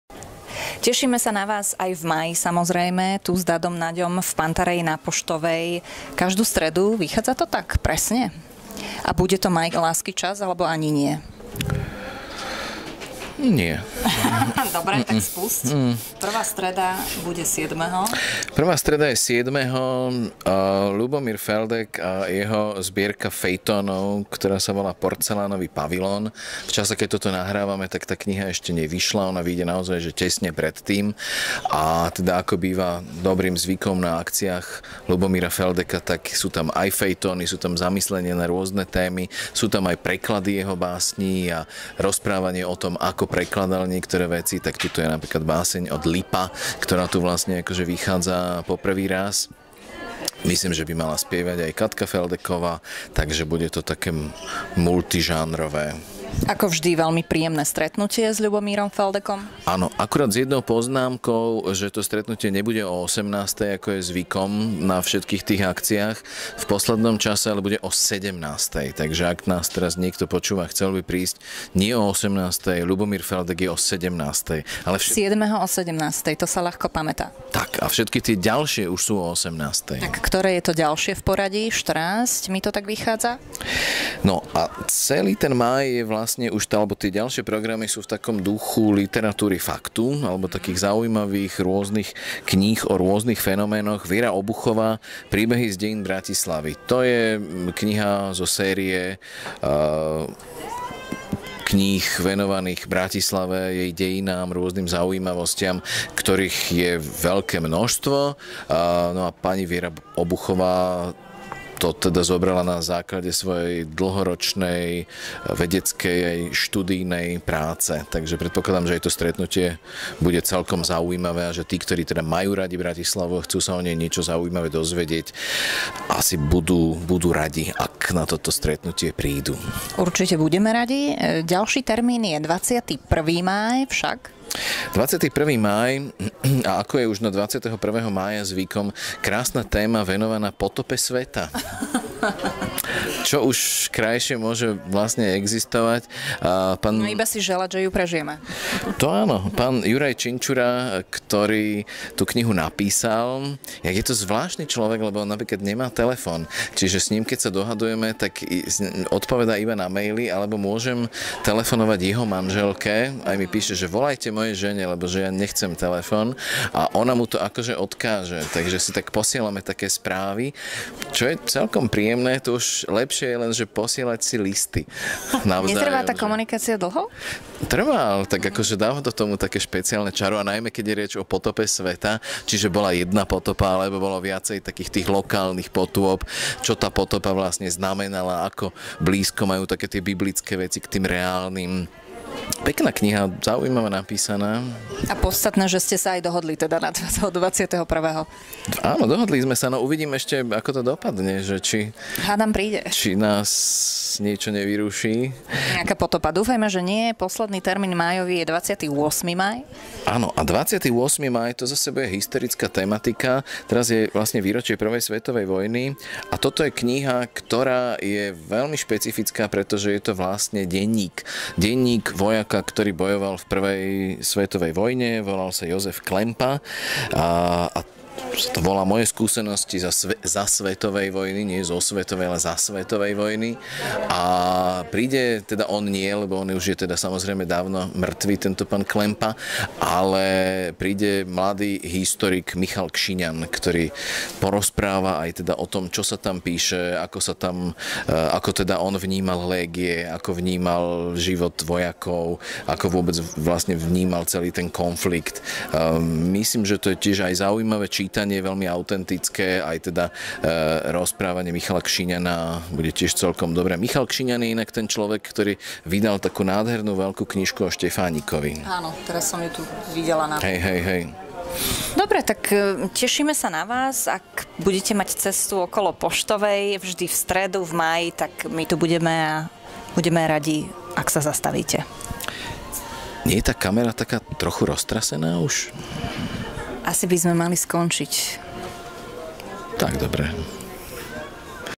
Dado Nagy: Májové talkshows v Panta Rhei
Aj v máji urobil Dado Nagy skvelý výber hostí do svojich talkshows  - pozrite si video - niečo ešte tento mesiac stihnete :-)